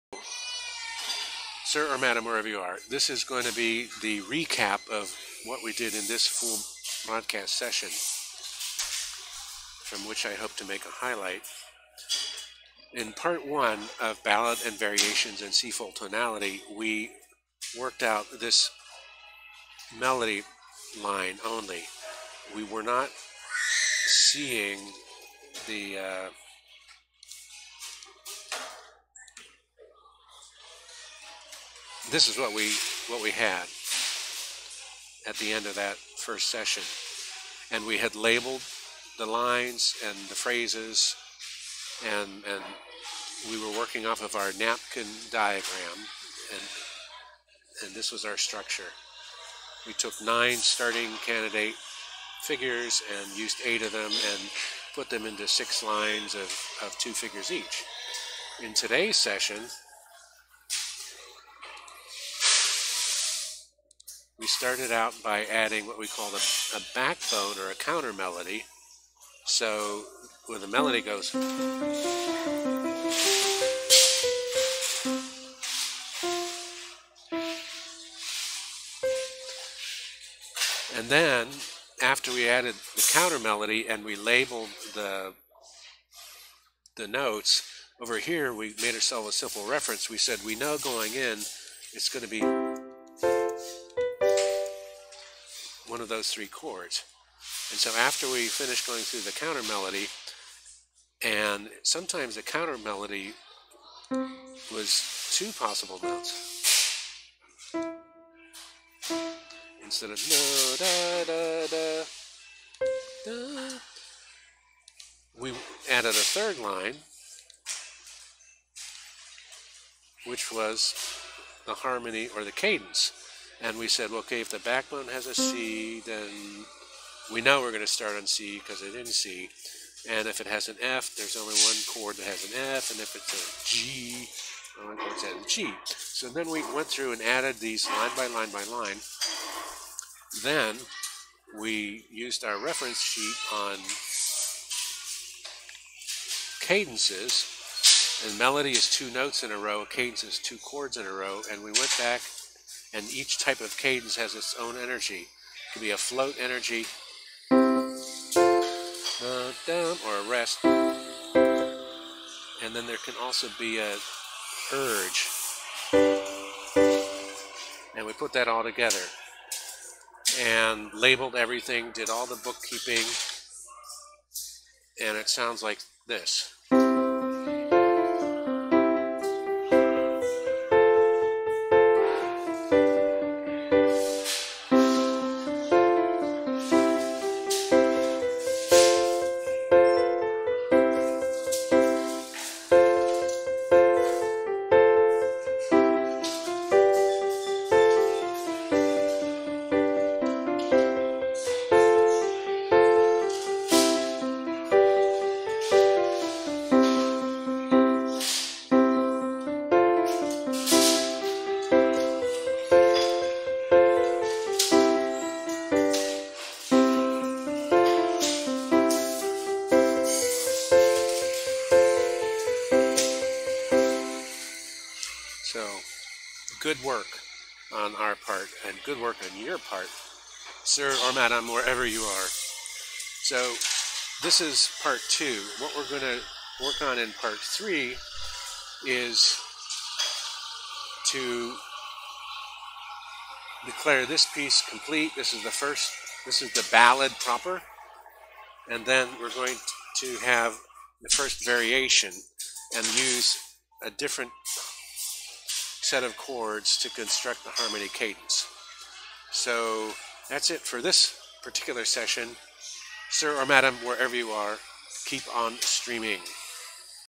Composing a Ballad Part 2 HARMONY Stream Highlight 15 Feb 2020
harmonic backbones and cadences are added to each phrase of a twelve-phrase song